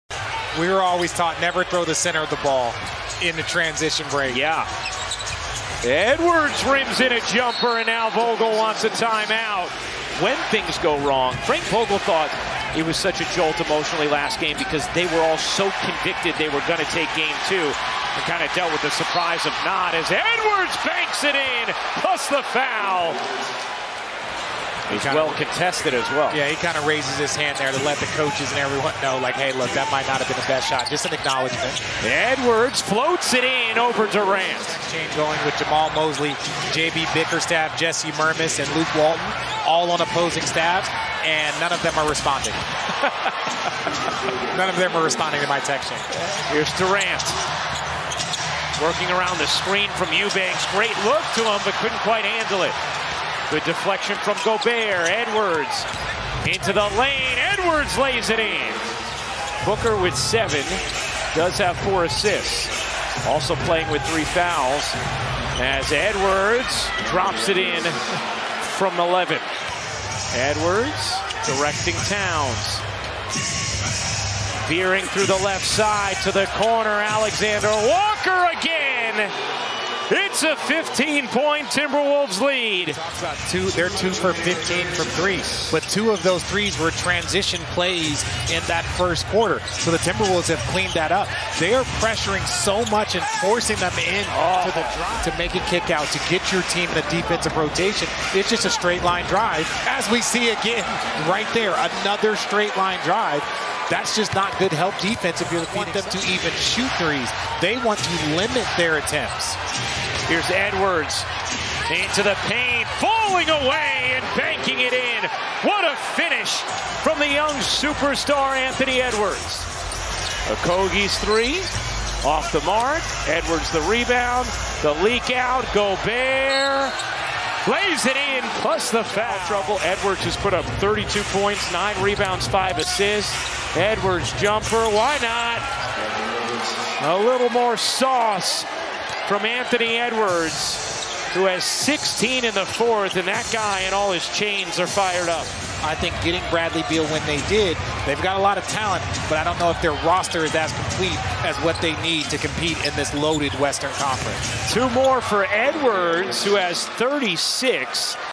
Anthony Edwards delivers as the @Timberwolves pick up the road win and take a 3-0 series lead!
Anthony Edwards delivers as the @Timberwolves
 pick up the road win and take a 3-0 series lead!

🐜 36 PTS
🐜 9 REB
🐜 5 AST

This is Ant's 7th career 30-point game in the postseason, tying Kevin Garnett for must such games in franchise history.